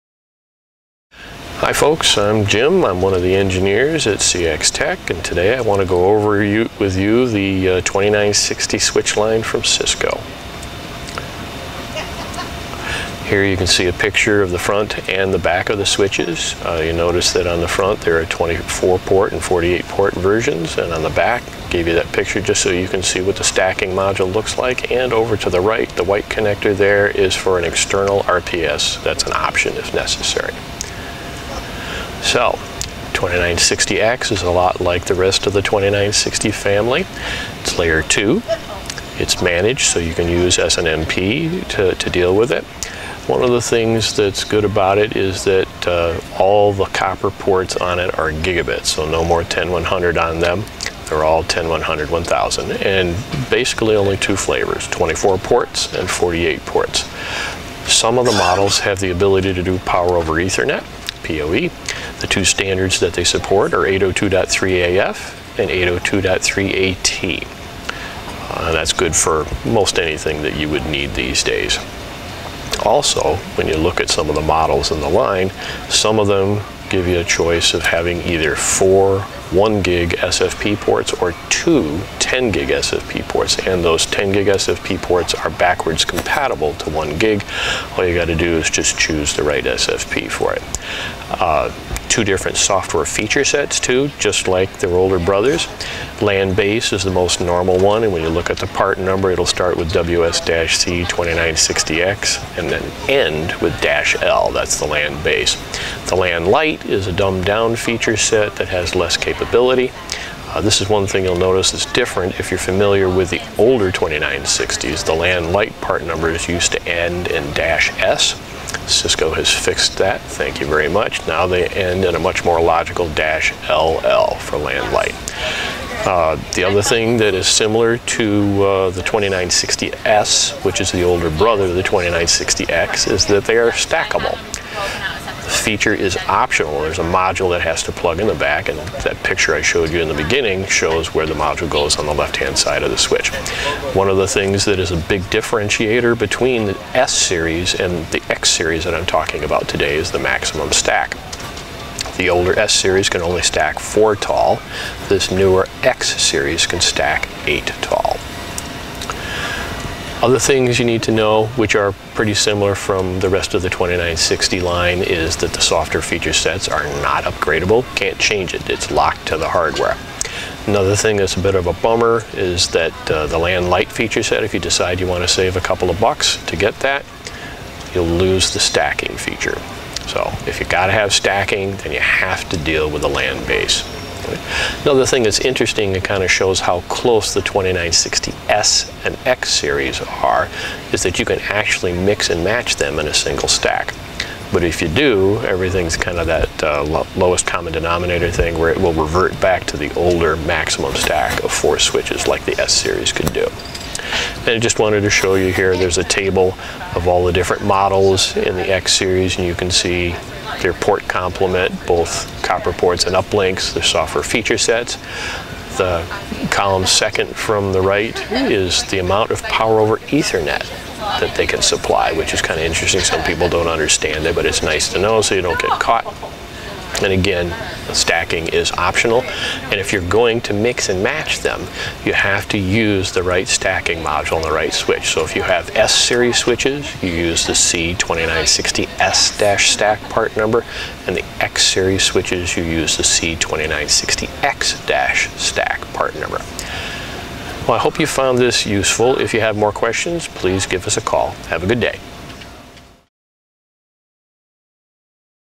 Cisco 2960-X Features and Capabilities - CXtec tec Tips
Jim, CXtec Product Engineer, explains some features and capabilities of the Cisco 2960-X Series switch line, including port counts, PoE standards, software sets, stacking considerations, and more.